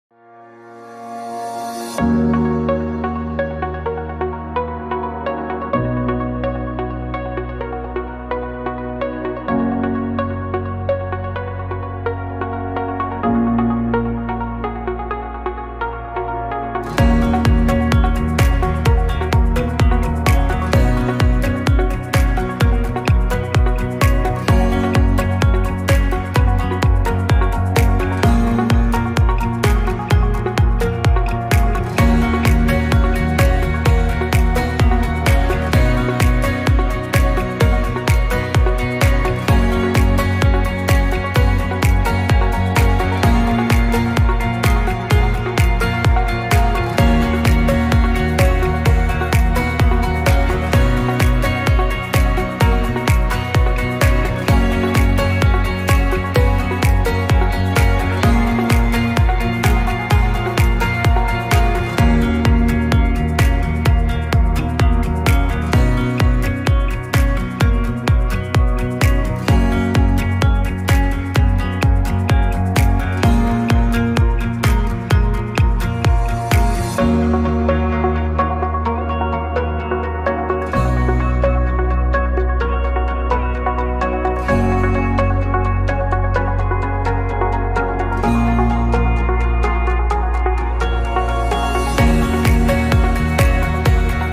Take care of the surroundings of the house, garages, street intersections, # VStarcam camera CS68-X5
Take care of the surroundings of the house, garages, street intersections, # VStarcam high-definition zoom camera CS68-X5, wifi wireless connection, 5x optical zoom, details are clearly visible, humanoid detection, IP66 waterproof, bidirectional pan tilt, stable and durable, click on the official website of Weishi Dakang to learn more!